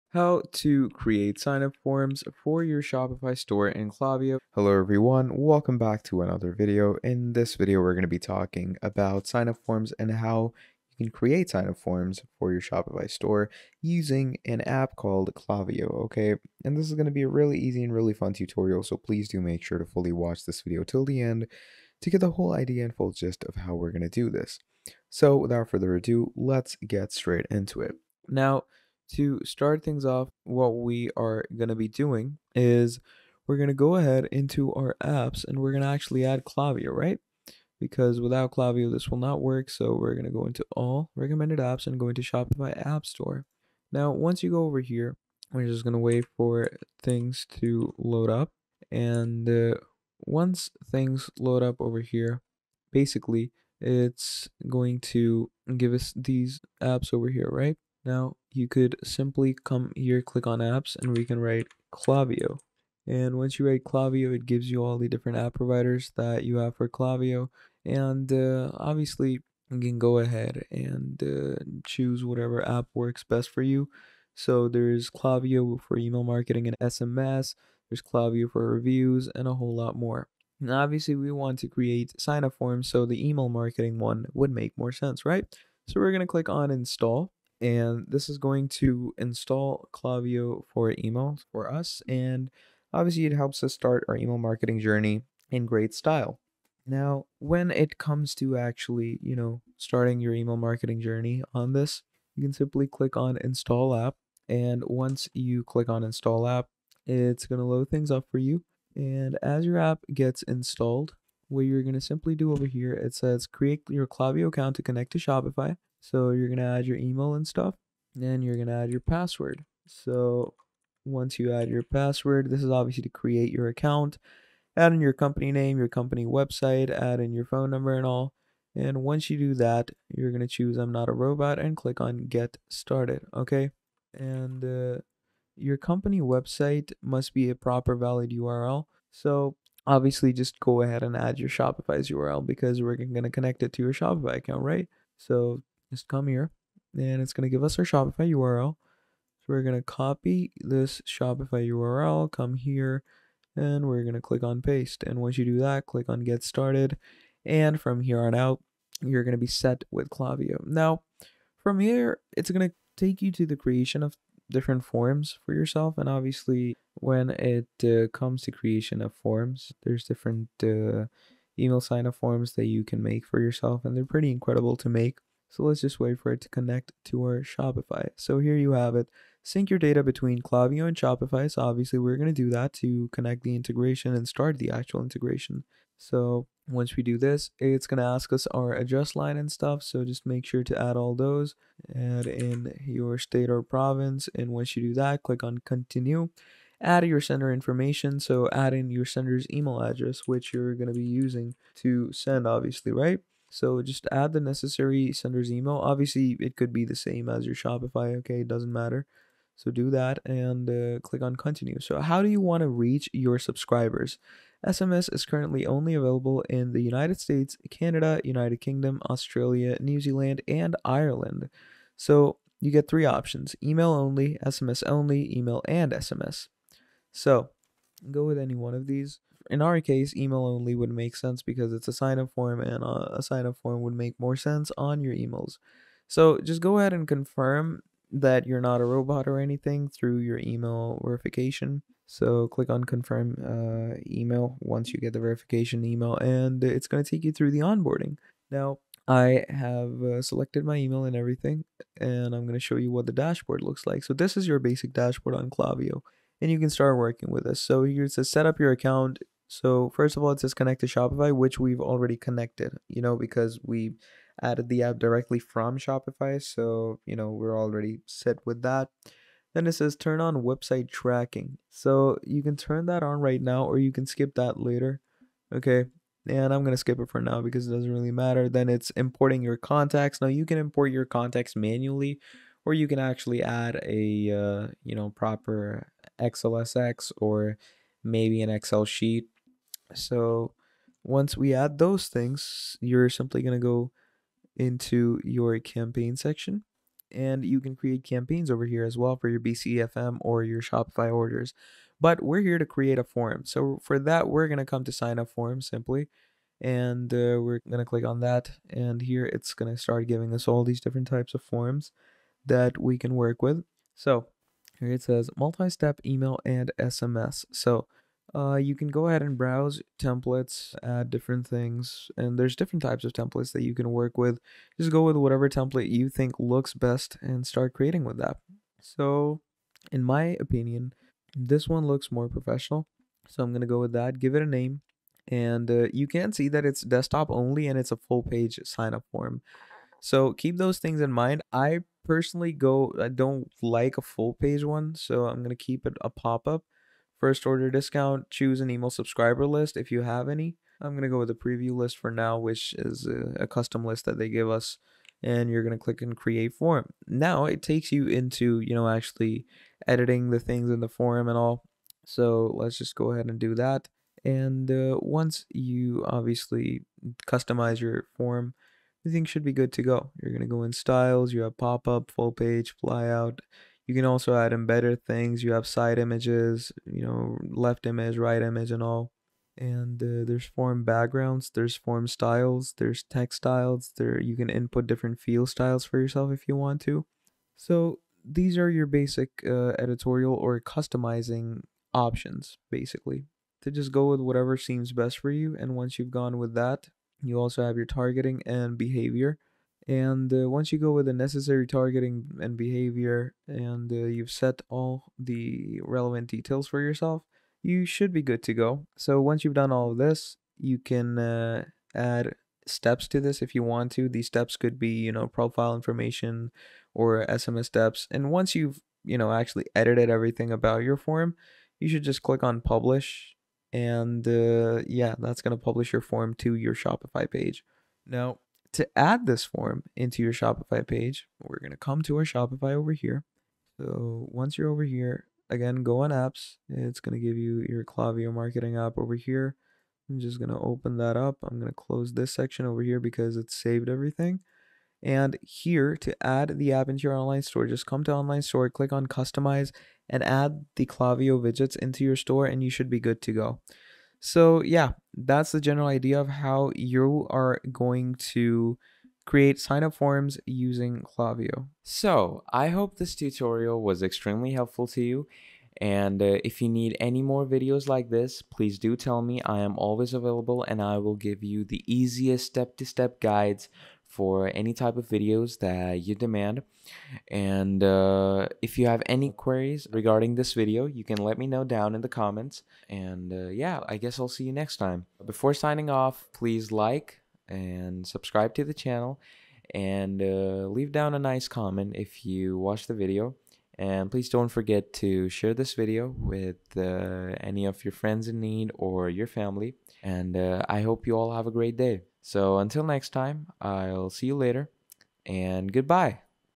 How to Create Signup Forms for your Shopify store in Klaviyo (2026)
Creating signup forms for your Shopify store in Klaviyo is a simple and effective way to grow your customer base and increase sales. To get started, first log into your Klaviyo account and go to the "Lists & Segments" tab. From there, click on "Create List" and choose "Shopify" as your integration. Next, select the "Signup Forms" option and choose a template that best fits your brand and style.

Customize the form with your own branding and fields to collect the necessary information from your customers. Once done, simply copy and paste the code into your Shopify store and start collecting valuable subscriber information. With Klaviyo's powerful segmentation and automation features, you can easily target and engage your subscribers, leading to higher conversions and customer retention.